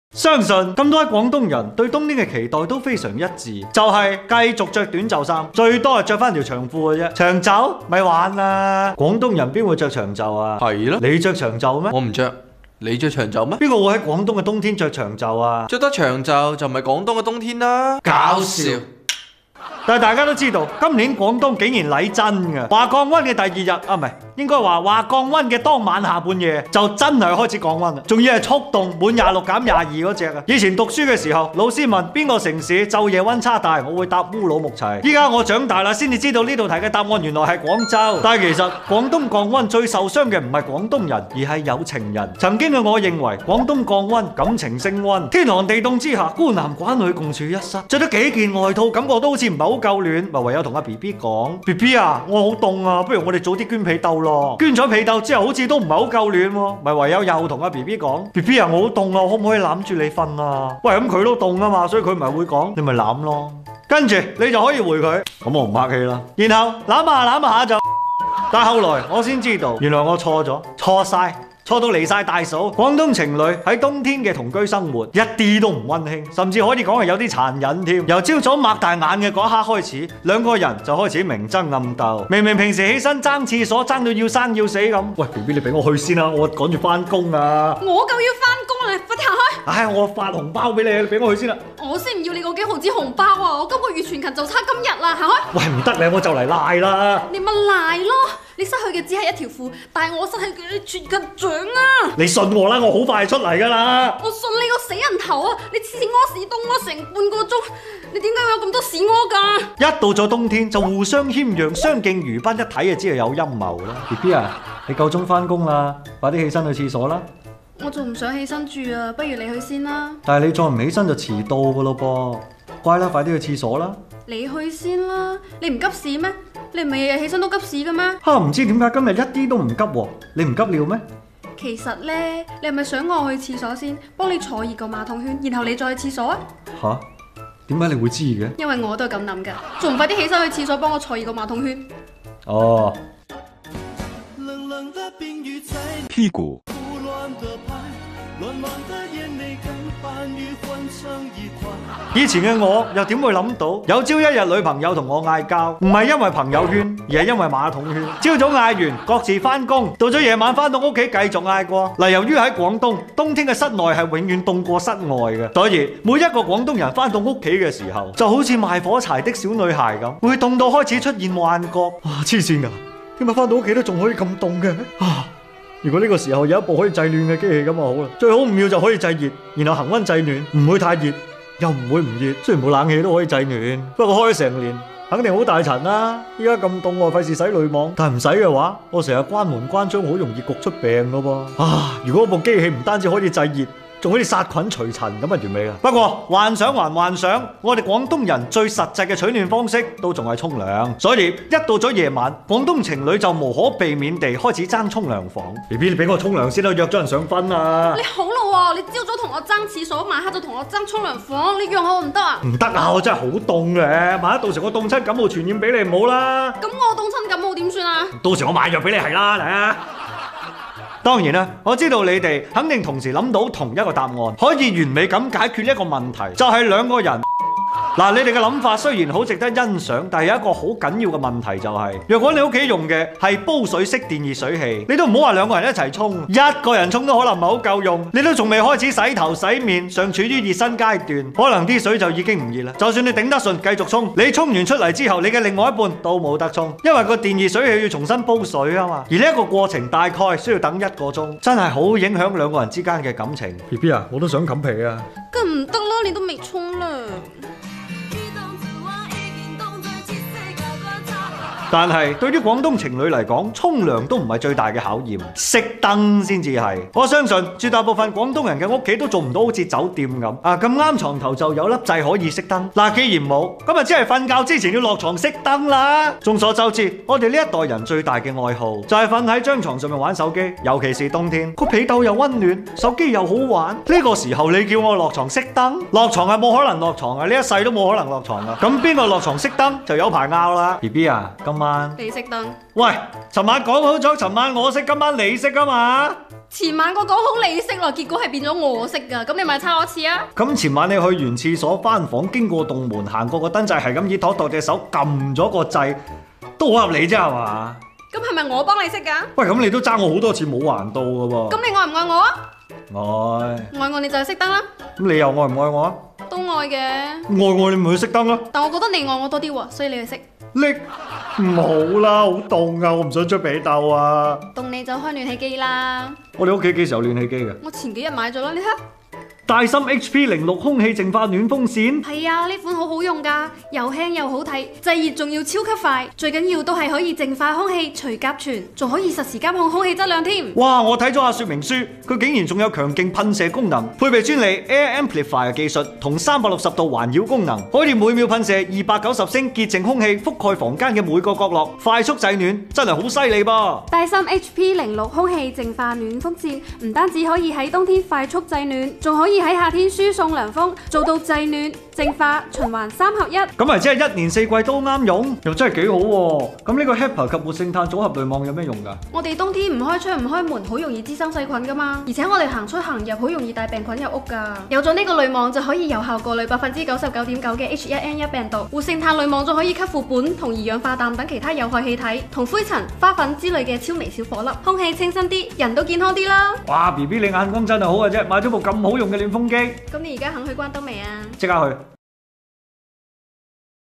【粵知一二】經歷過廣東冬天，我才明白談一場不分手的戀愛有多難！
「第1369期」經歷過廣東冬天，我才明白談一場不分手的戀愛有多難！
有對象就能在廣東溫暖過冬？那你是太天真了！